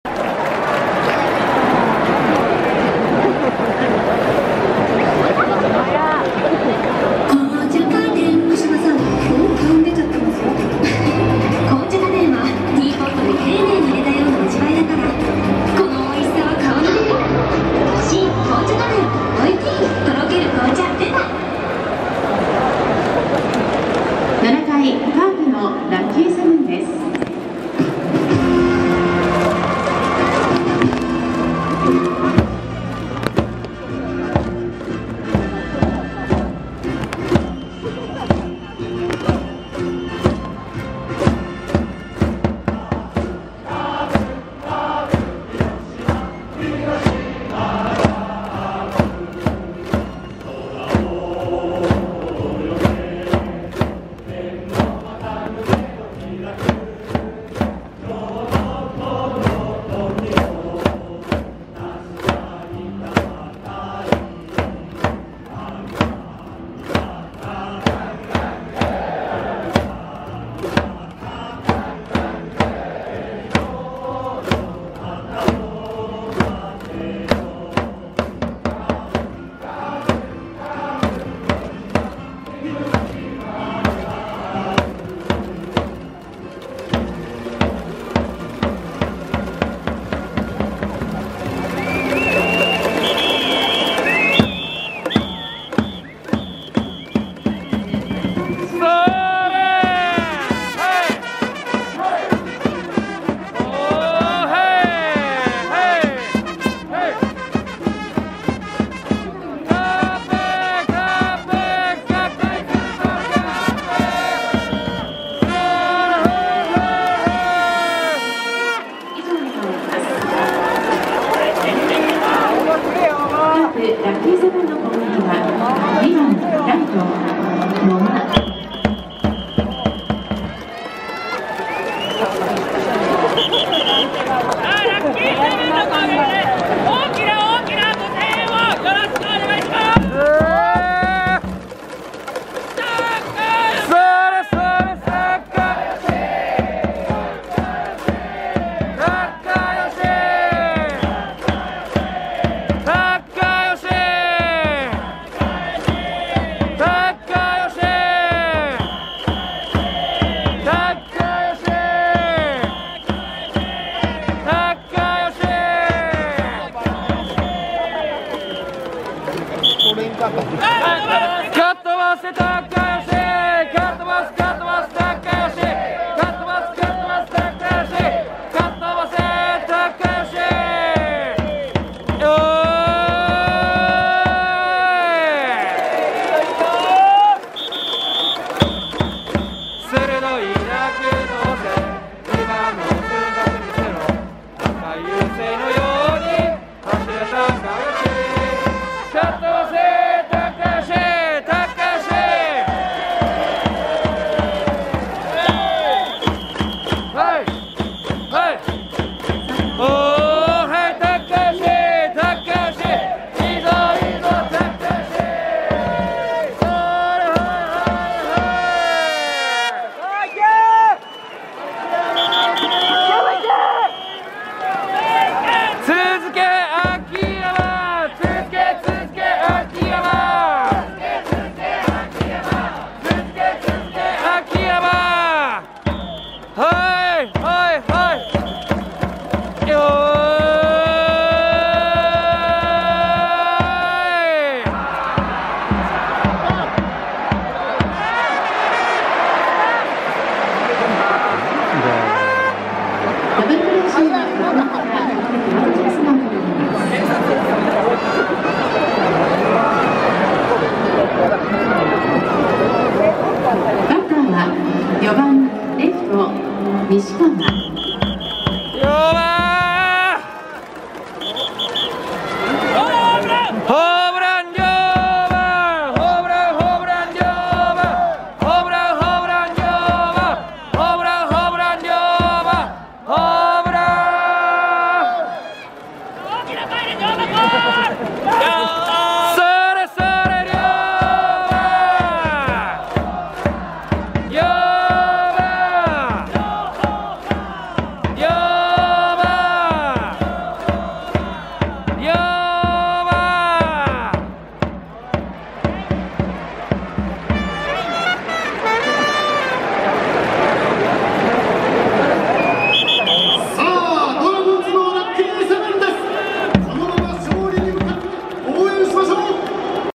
カープ外野ビジター🎏ラッキー7️⃣ 負けられん逆転するぞカープ❗💪 野間峻祥⇒秋山翔吾⇒西川龍馬 2023.7.7対中日ドラゴンズ
勝利を我らに！Let's Win